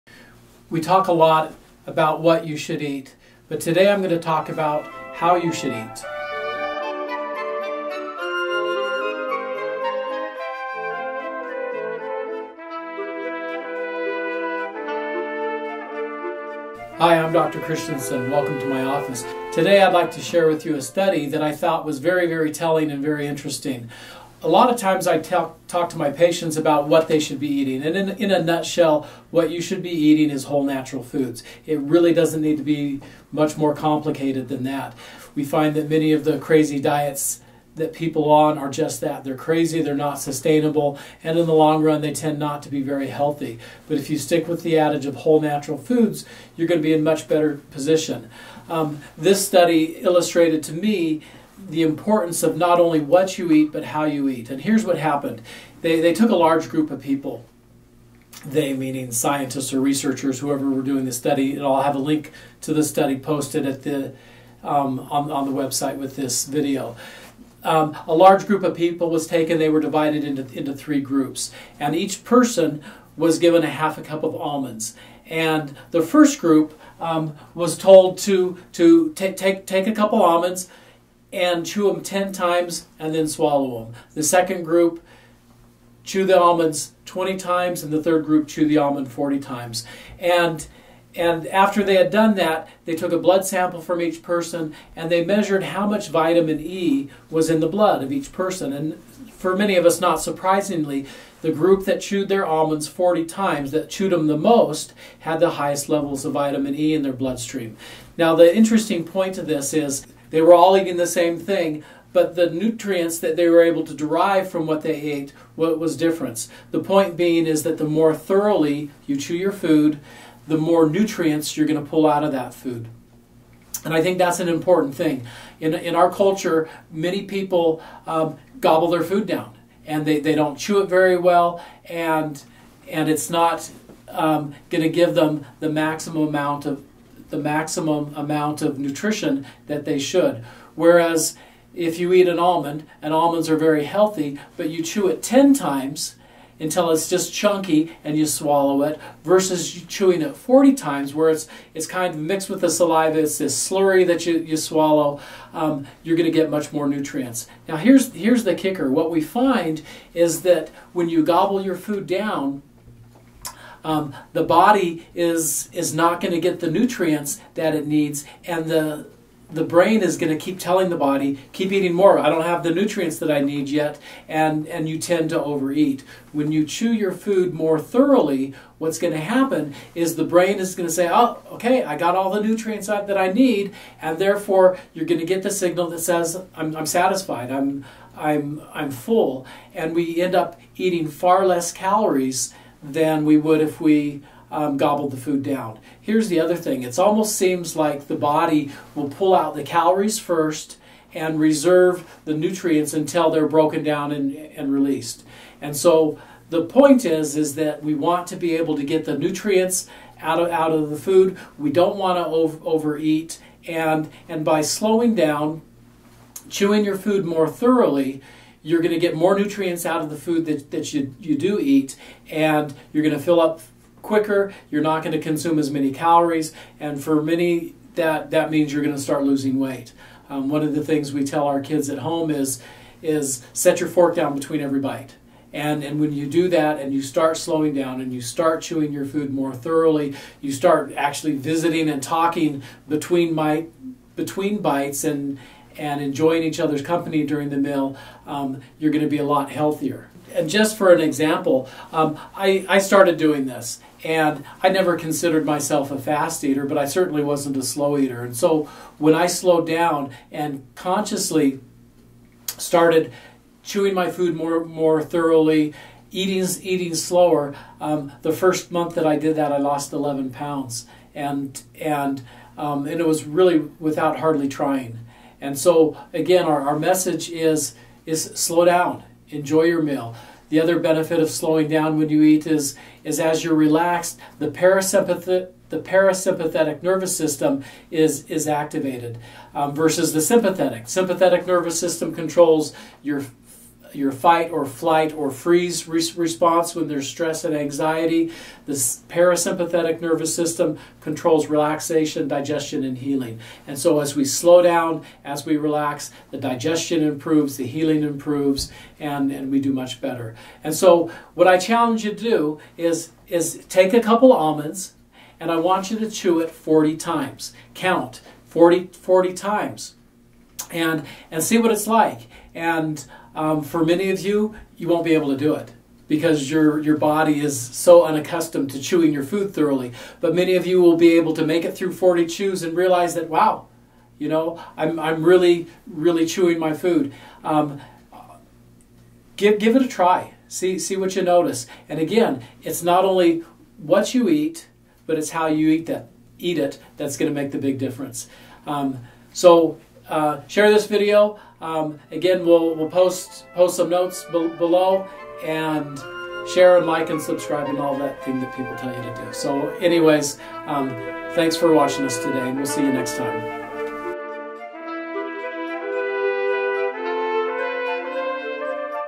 How to Eat Less, Get more from your food, and Lose Weight at the Same Time
Dr. Christensen goes over the importance of paying attention to how you eat, in addition to what you eat.